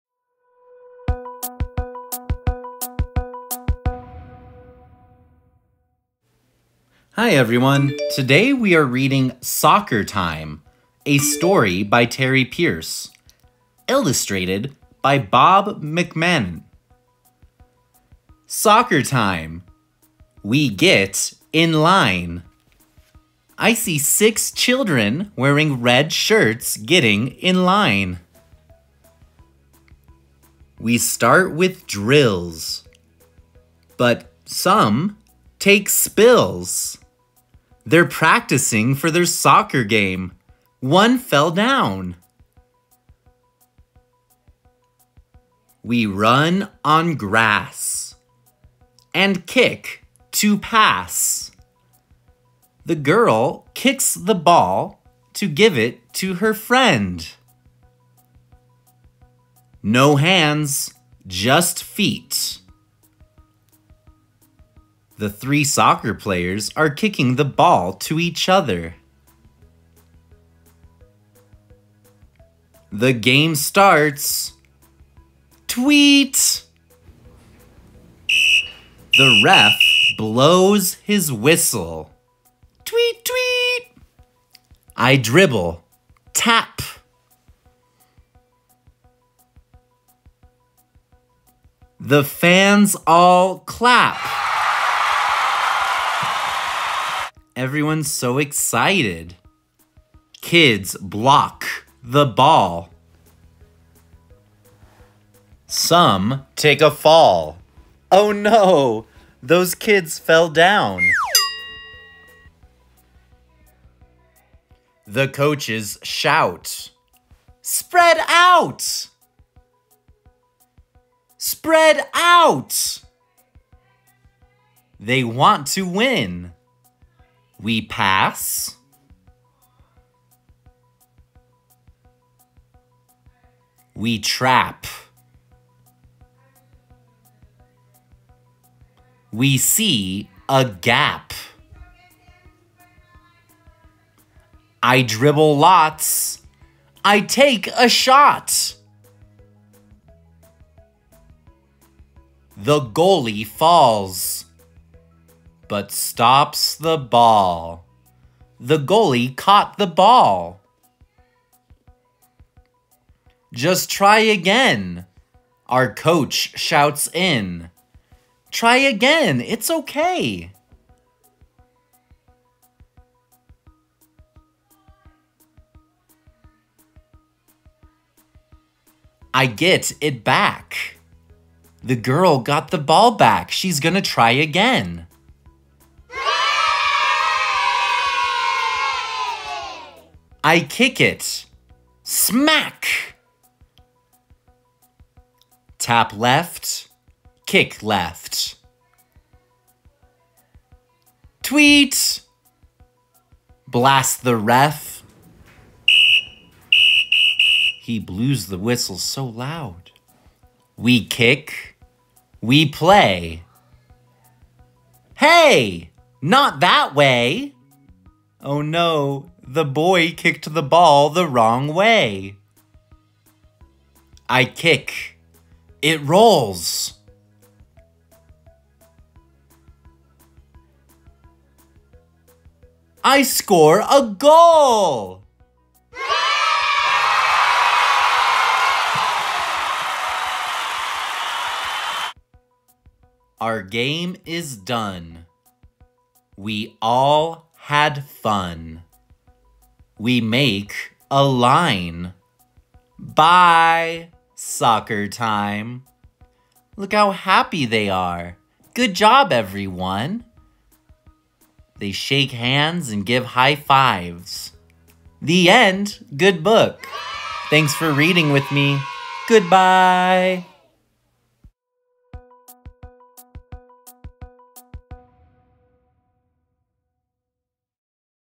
BOOK: SOCCER TIME - by Terry Pierce -Reading Level 1
Soccer Time, Written by Terry Pierce and Illustrated by Bob McMahon. Read by Trey Pierson. Part of the Ready to Read, Step into Reading Series.